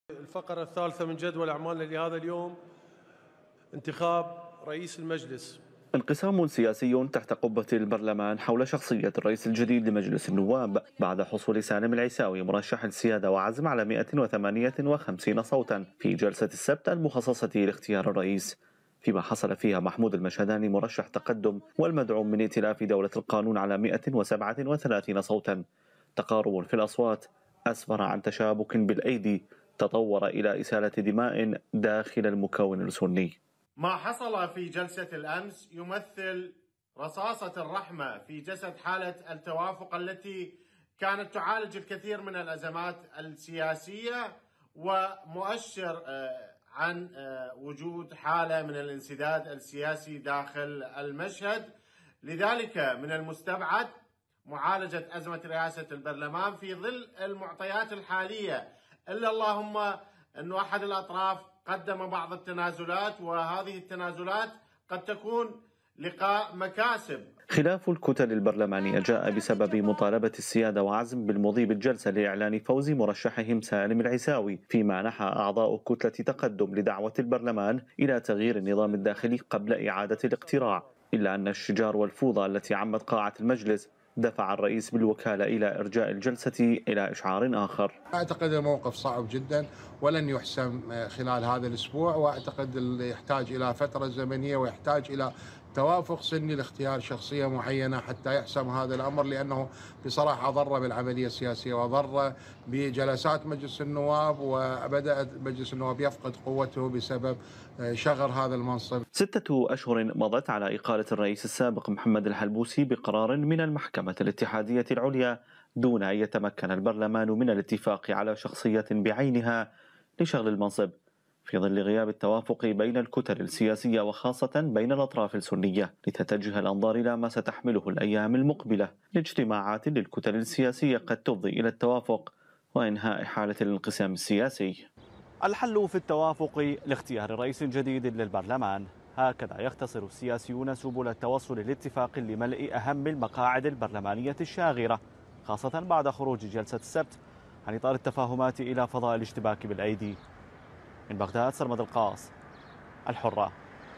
انقسام سياسي يفضي إلى اشتباكات بين أعضاء في مجلس النواب
رفع مجلس النواب جلسته 25 من الفصل التشريعي الأول إلى إشعار آخر، بسبب عدم التوصل إلى اتفاق بين كتلتي السيادة وعزم من جهة وكتلة تقدم من جهة أخرى على مرشح لمنصب رئيس البرلمان، خلاف وصل حد الاشتباك بالأيدي.

حمل تطبيق "الحرة كونيكت" للتلفزيونات والهواتف الذكية:

أمازون فاير